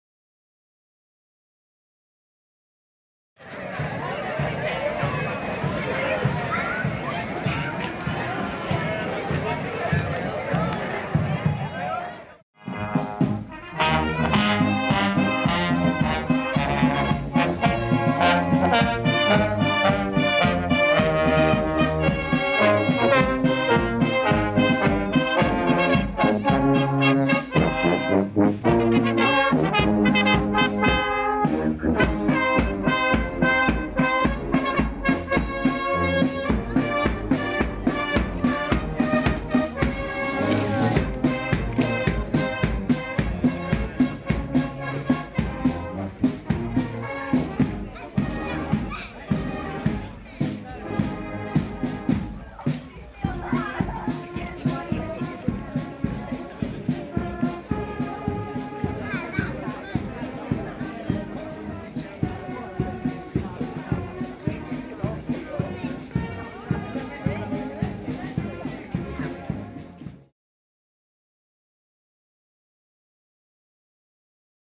Buckley Jubilee 2019 - Flintshire, North Wales
Hundreds of people turned out to enjoy Buckley Jubilee in Flintshire, North Wales, despite gloomy weather conditions.

The 163rd annual event is organised by 11 churches in the town and the surrounding areas.